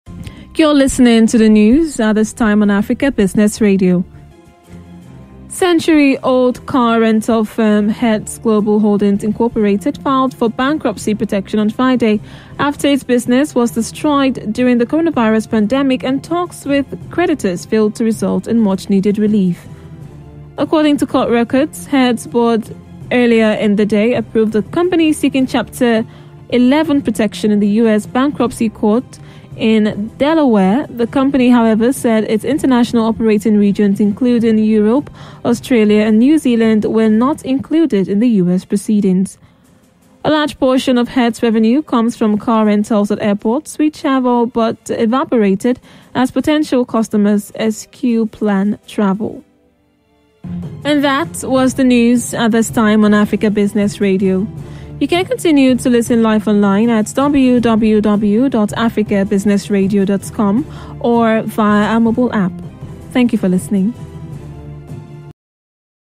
Hertz Files for U.S. Bankruptcy Protection
Century old car rental firm Hertz Global Holdings Inc filed for bankruptcy protection on Friday after its business was destroyed during the coronavirus pandemic and talks with creditors failed to result in much needed relief.
According to court records, Hertz’s board earlier in the day approved the company seeking Chapter 11 protection in a U.S. bankruptcy court in Delaware. The company however said Its international operating regions including Europe, Australia and New Zealand were not included in the U.S. proceedings.
A large portion of Hertz’s revenue comes from car rentals at airports, which have all but evaporated as potential customers eschew plane travel.